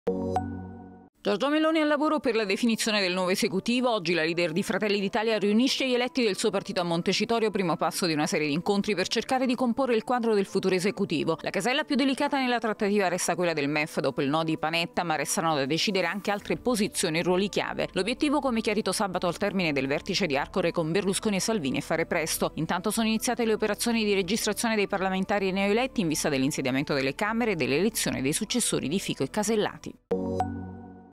Governo, Meloni al lavoro per definire il nuovo esecutivo
Oggi riunione a Montecitorio tra gli eletti di Fratelli d'Italia.